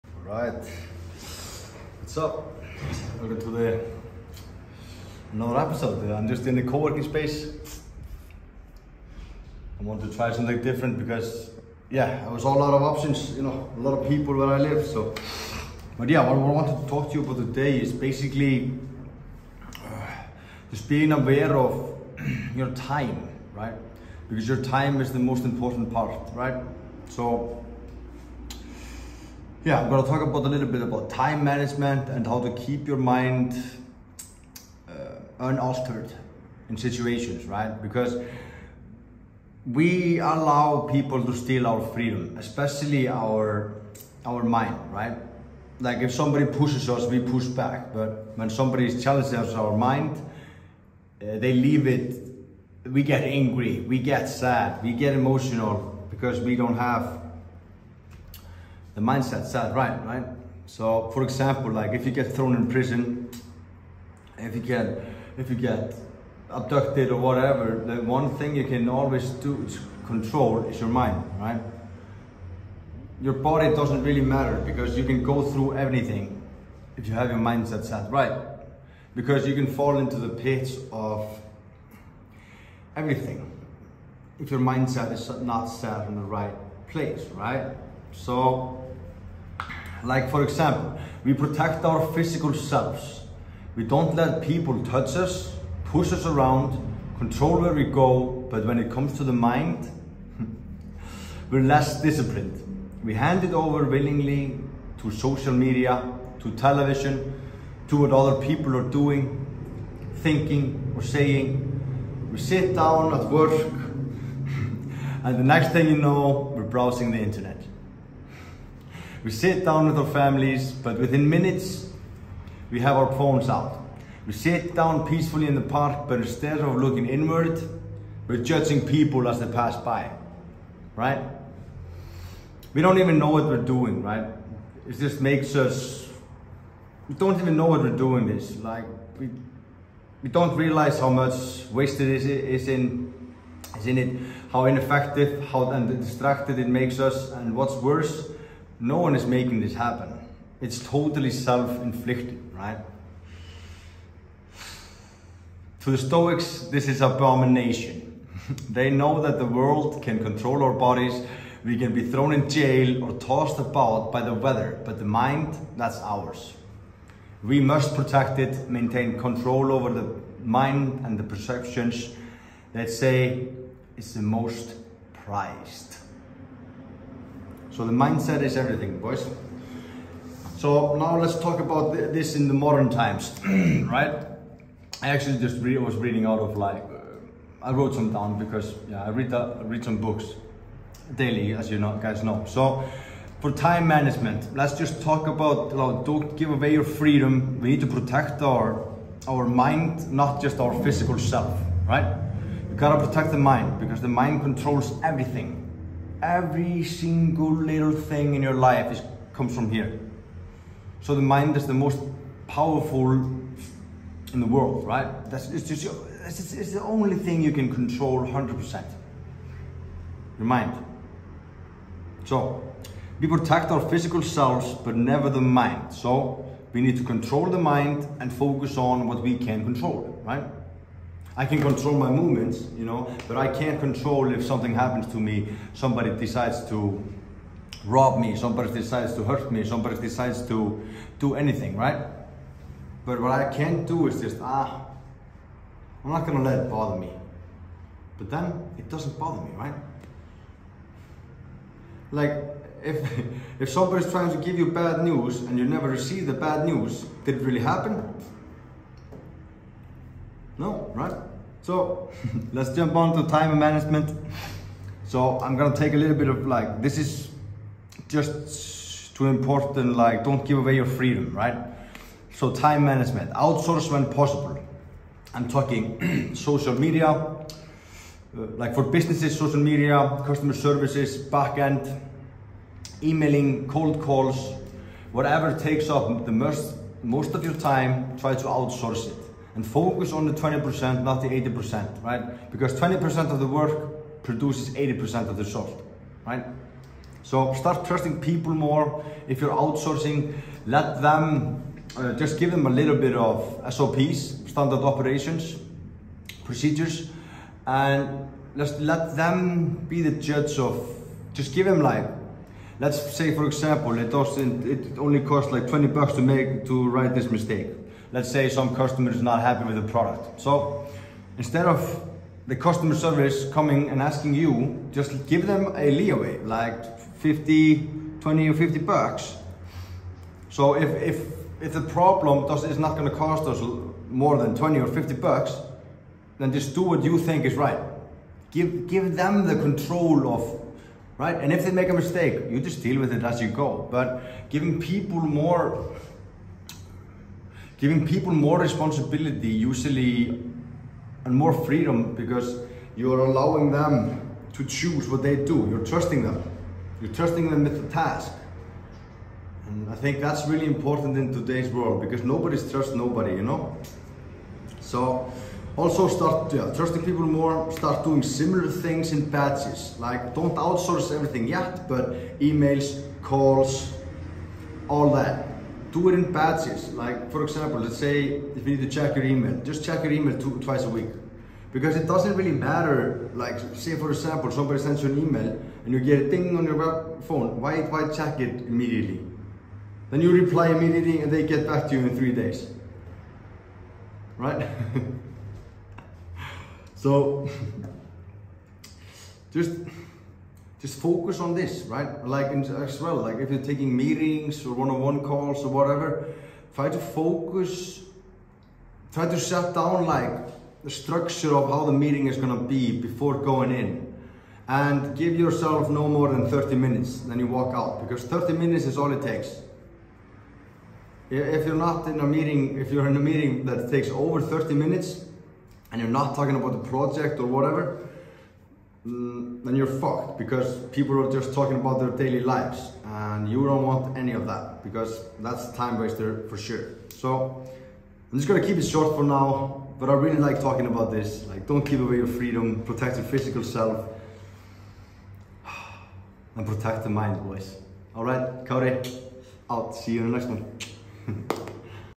Stop giving away your Freedom
Stop allowing people to take away your mental freedom, we would never allow anyone to treat our bodies like this, if someone pushes us we push back.